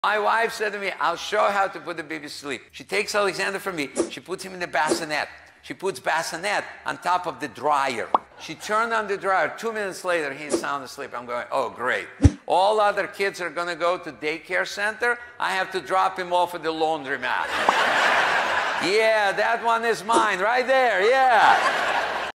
My Wife has the BEST Sleep Hack for the Baby 💤 😴 | Yakov Smirnoff
Enjoy this classic joke about my wife's AMAZING sleep hack for the baby!